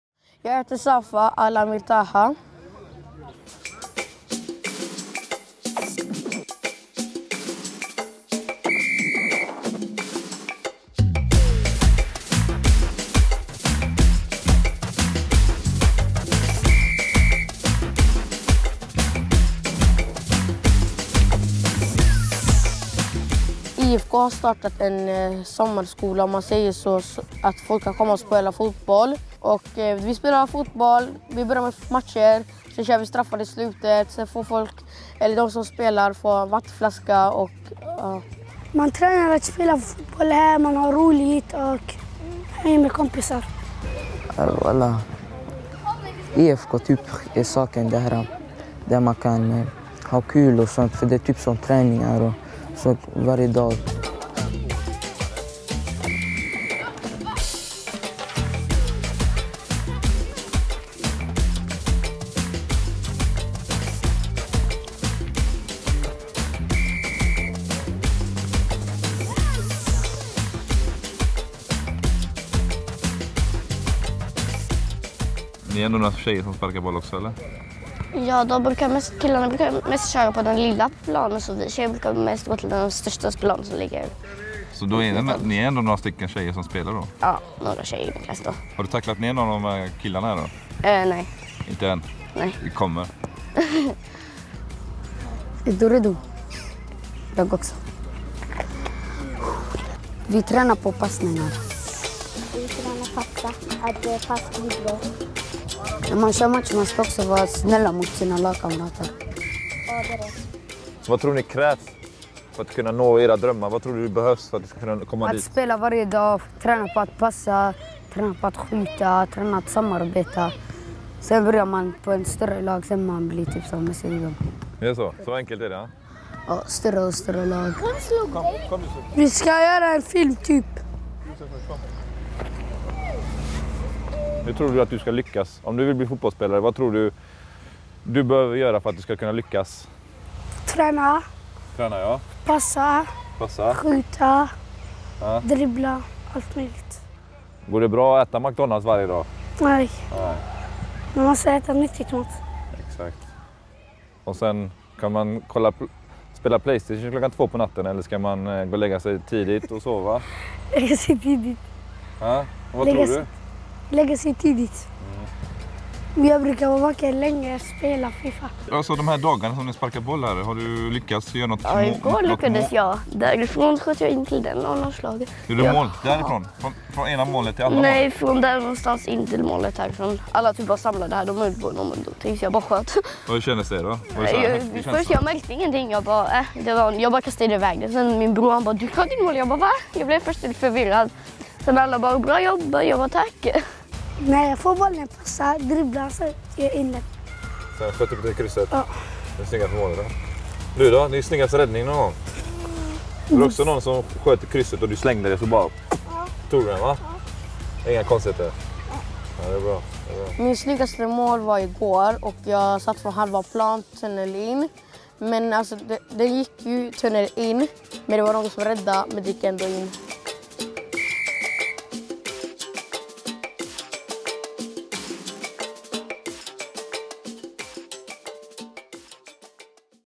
Planer för framtiden avsnitt 2: Barnen
Planer för framtiden. Så heter en av IFK Göteborgs mest spännande satsningar någonsin. I samarbete med Johan Cruyff Foundation anlägger IFK Göteborg fotbollsplaner i Göteborgs förorter. Vi gör det för att ge Göteborgs barn och ungdomar bättre förutsättningar. För att känna av betydelsen av denna satsning ger IFK Göteborg ut en miniserie i fyra delar. Där tar vi rygg på fotbollsspelaren Martin Smedberg-Dalence och följer med till hans Hammarkullen. Vi börjar från början. Med uppväxt och vänner. Sen möter vi framtiden och tar del av barnens tankar och energi. Planer för Framtiden består av fyra avsnitt och visas på IFK Göteborgs hemsida, Facebook och YouTube-kanal.

Avsnitt 1: Vännerna
Avsnitt 2: Barnen
Avsnitt 3: Familjen
Avsnitt 4: Musiken

Idé och regi: Dragster Kommunikation AB
Produktionsbolag: Pensky